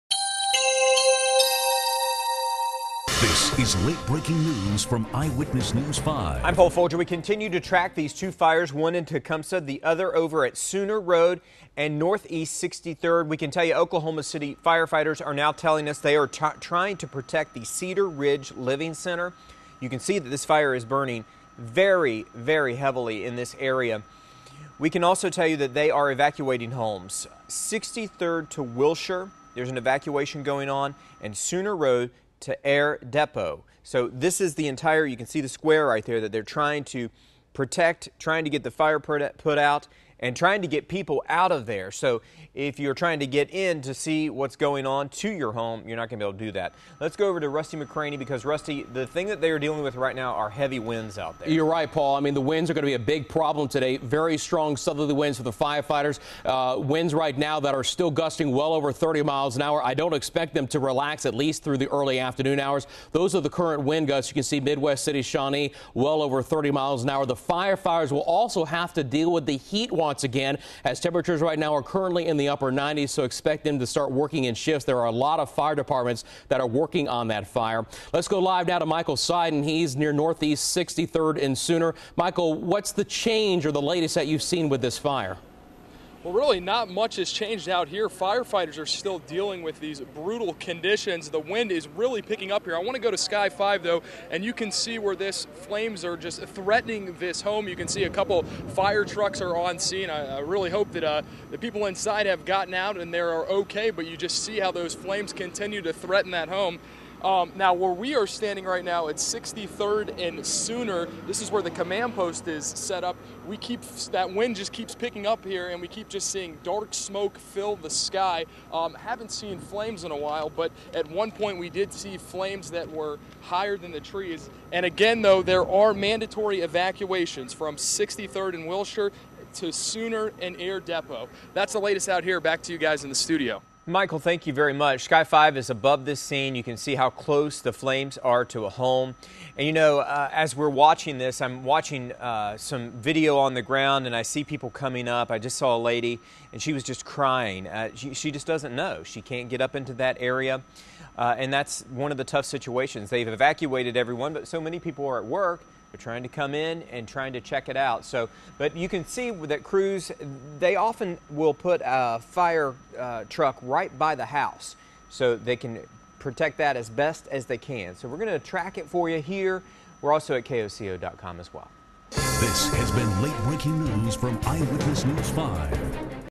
Evacuations Ordered Due To OKC Wildfire
People in the vicinity of a large wildfire in northeast Oklahoma City are being ordered to evacuate on Tuesday afternoon.